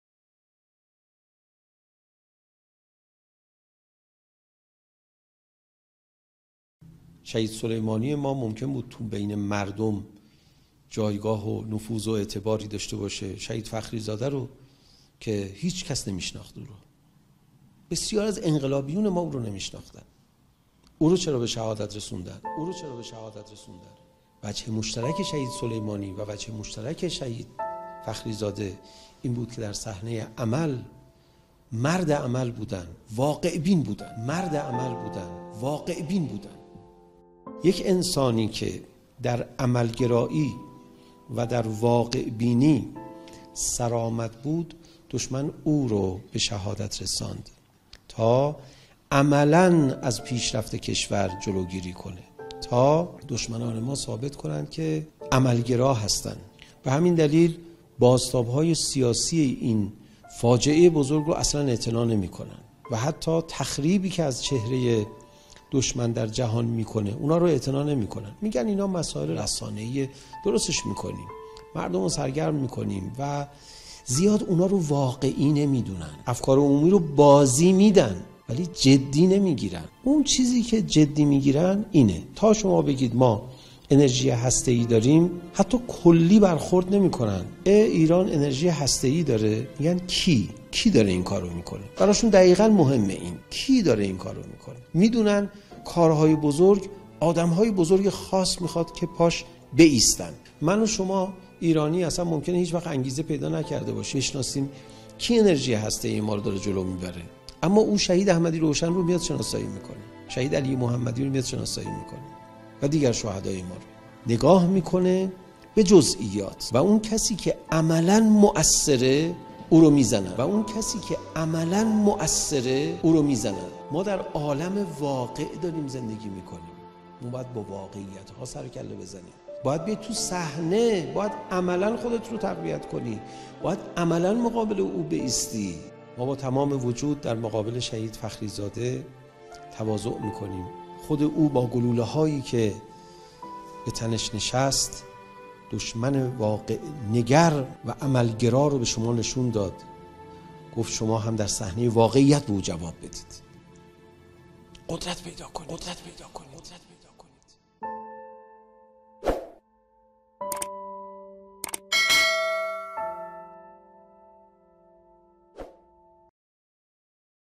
دشمن اسلام کی نگاہ - Agha Ali Raza Panahian (Urdu)
دشمن اسلام کی نگاہ میں
شہید سلیمانی کا ہوسکتا ہے لوگوں میں مقام و منزلت و نفوز ہولیکن شہید فخری زادہ کو تو کوئی نہیں جانتا تھا۔ ان کو کیوں شہید کیا؟
از کلام حجت الاسلام آغا علی رضا پناھیان